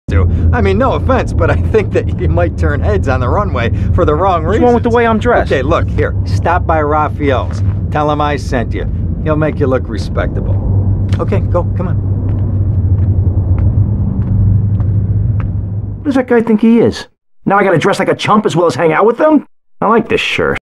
2 Toms in 1 video
And here's Tom stuck in the door LoL..Because whoever made the mod patially failed in doing it properly so you can see 2 Toms in 1 video..